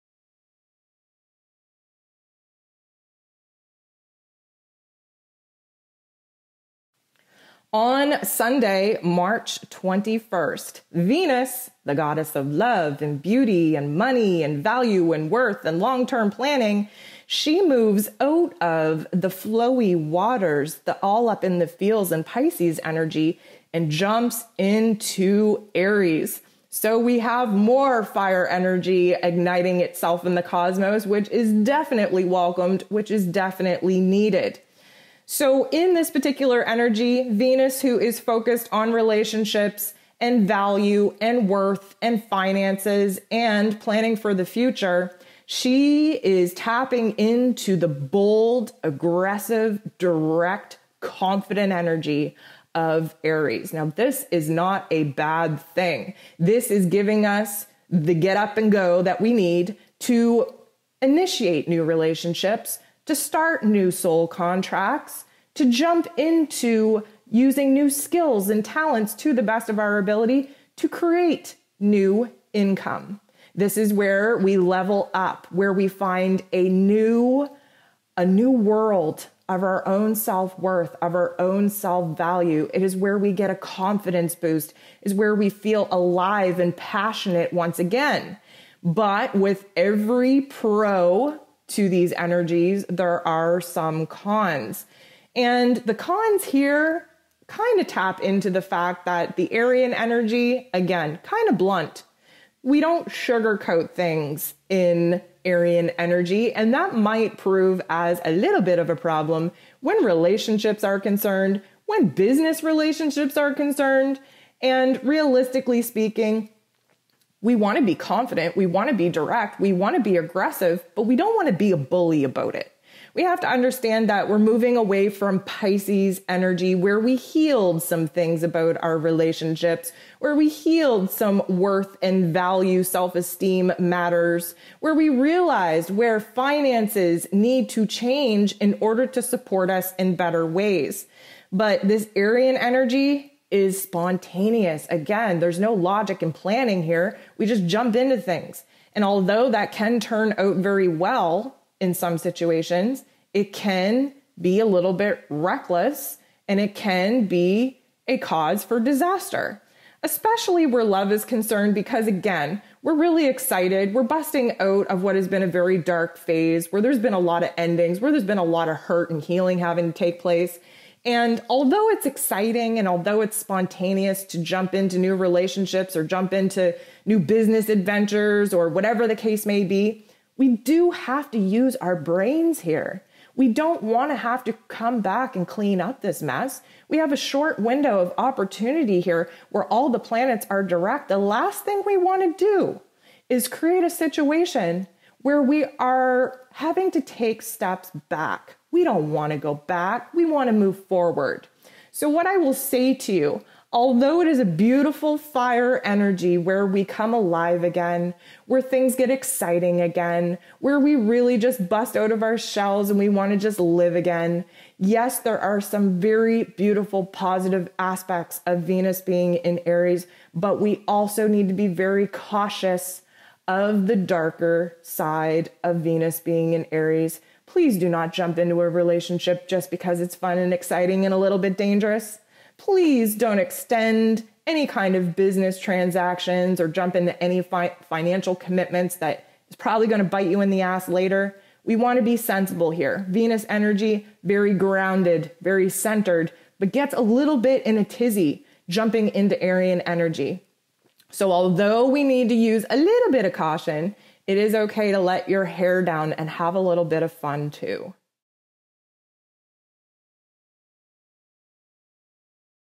March's Energy Forecast: Mar. 21st - Venus in Aries
This month's energy forecast broken down one cosmic event at a time. We cover the energy based off of astrology, the Schumann Resonance, and the journey of ascension. Raising our vibration one shift at a time.